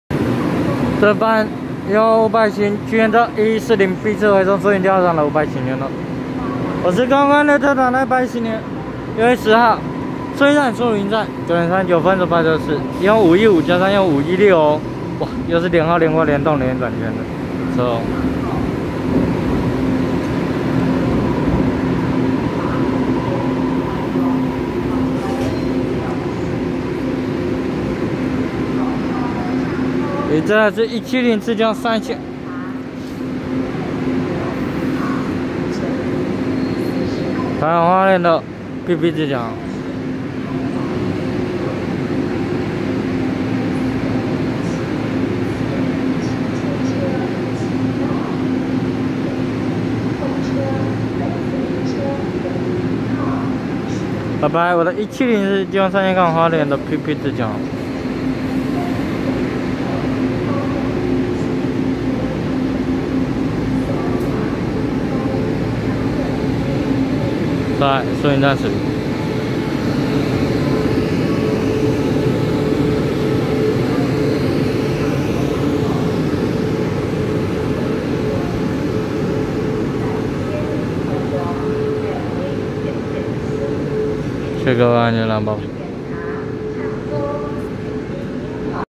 1140B次迴送樹林調車場的EMU515+EMU516連號連掛連動聯運轉區間車，和170次經由山線開往花蓮的PP自強號
110/6/10樹林站9:39分拍攝1140B次迴送樹林調車場的EMU515+EMU516連號連掛連動聯運轉區間車，和170次經由山線開往花蓮的PP自強號